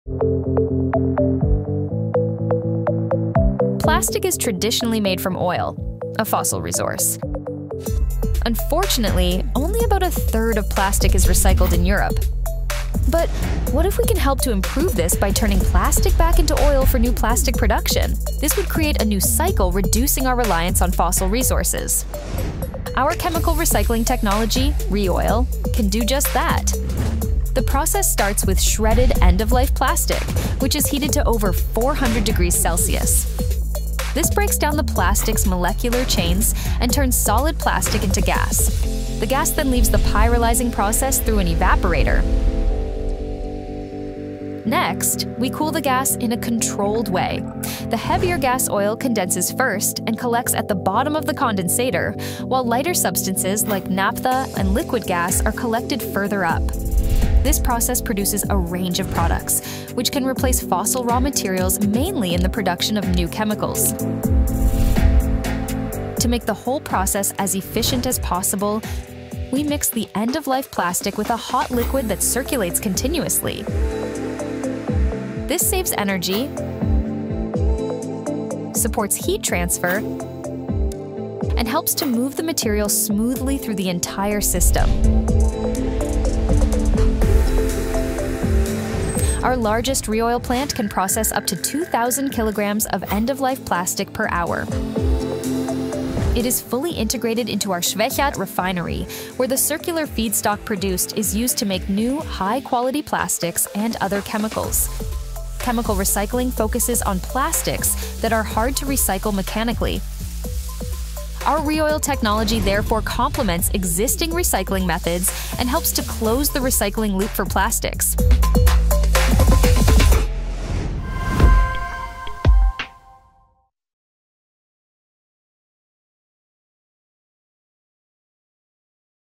ReOil®: Transforming End-of-life Plastics into Circular Solutions
Discover how our proprietary ReOil® technology is revolutionizing the way we think about plastics. By converting end-of-life plastics at into circular feedstock, ReOil® helps reduce reliance on fossil resources and supports the transition to a circular economy.

How does it work? These plastics are heated to over 400°C, producing pyrolysis oil. This oil is then further refined into base chemicals, offering a circular alternative to fossil resources in the production of chemicals, for example plastic.

With over 15 years of operational experience, ReOil® is one of the most advanced and scalable chemical recycling technologies on the market. Watch now to learn how we’re turning end-of-life plastics into circular solutions.